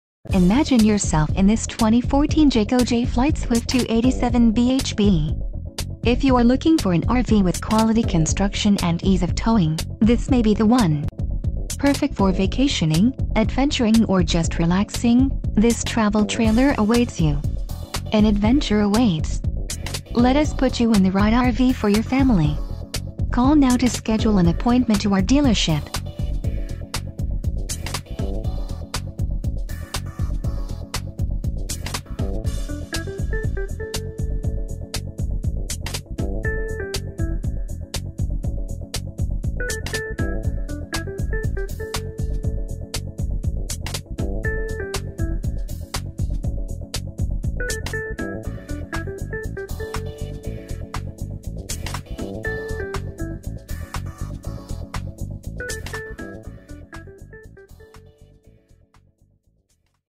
2014 Jayco Jay Flight Swift - Summit RV - Ashland, KY 41102
Always ready to hit the highway to adventure, this all-new 2014 Jay Flight manufactured by Jayco redefines versatility! Complete with a harvest interior and enough room for family and friends, this model 287BHBE travel trailer is designed to meet the needs of today\'s traveler. Come learn more for yourself today at our local Ashland, KY dealership! , Customer Value Pkg-13.5k A/c\r\nPower Pkg: Awning  and  Tongue Jack\r\nTechnology Pkg\r\nCustomer Value Pkg Disc\r\nFuel Surcharge\r\nVsp Disc